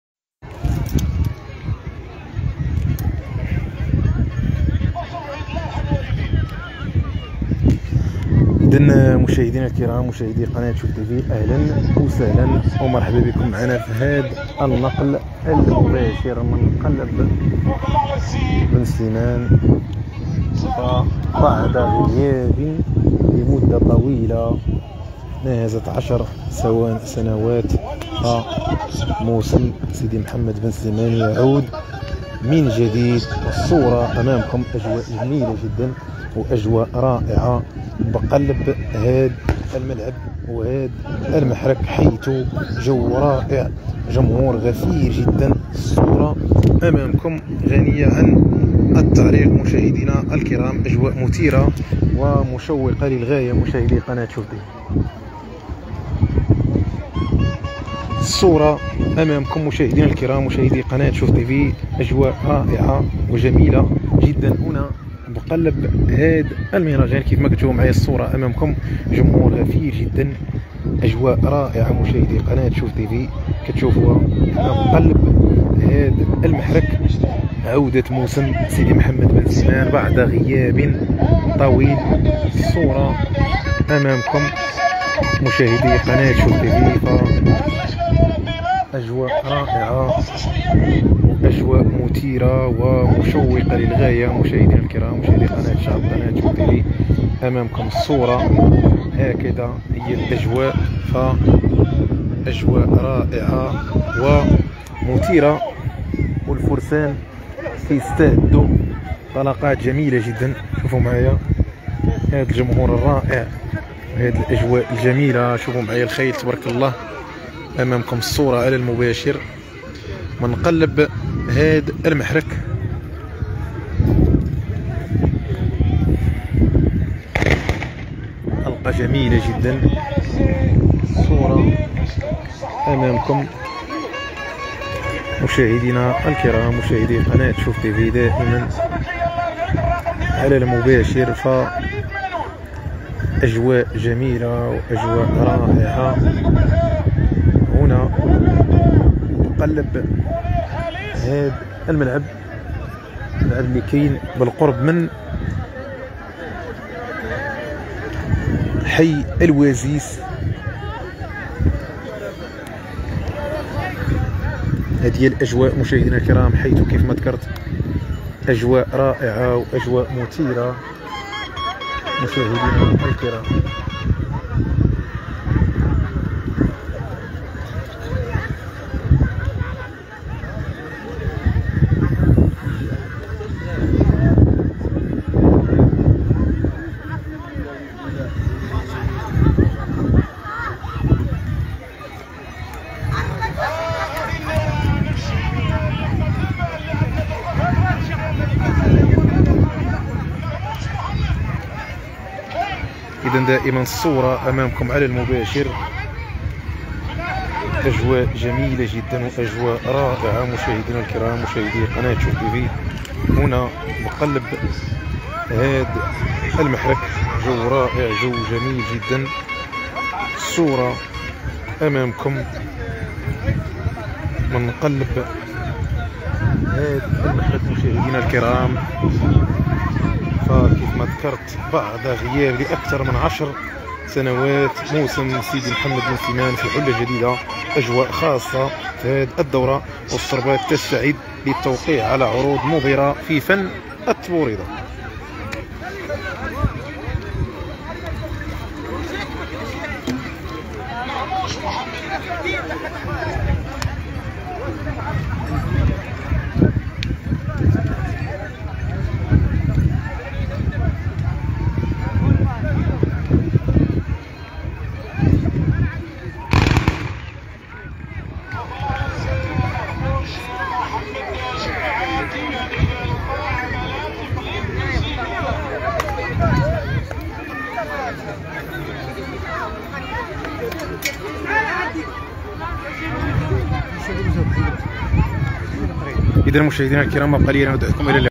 مباشرة من بنسليمان: بعد غيابه لأكثر من عشر سنوات موسم سيدي محمد بنسليمان في حلة جديدة
مباشرة من بنسليمان: بعد غيابه لأكثر من عشر سنوات موسم سيدي محمد بنسليمان في حلة جديدة و أجواء خاصة في هذه الدورة و السربات تستعد للتوقيع على عروض مبهرة في فن التبوريدة